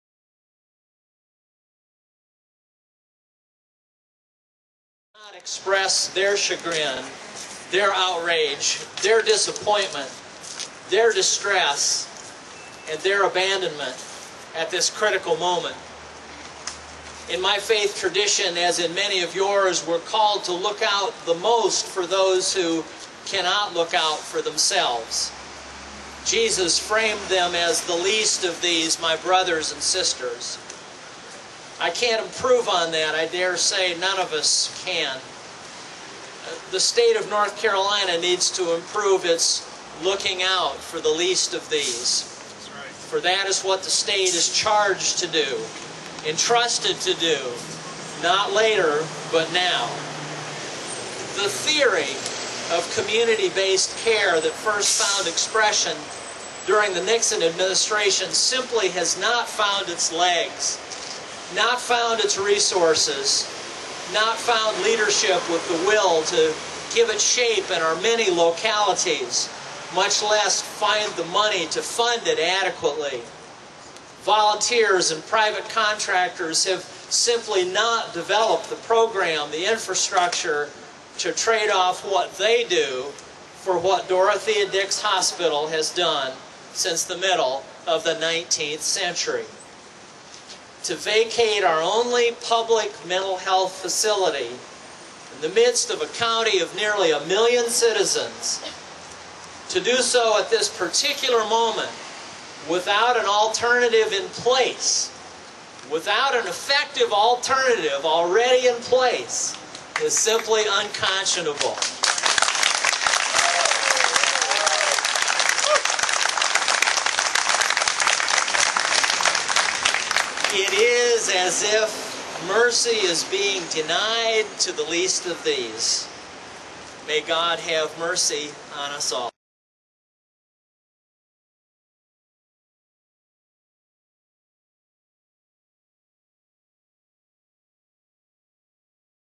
PastorSteveHickle.wmv
Rev. Steve Hickle, Pastor, Fairmont United Methodist Church,speaking at the "Rally to Save Dix," October 28, 2010, Raleigh, NC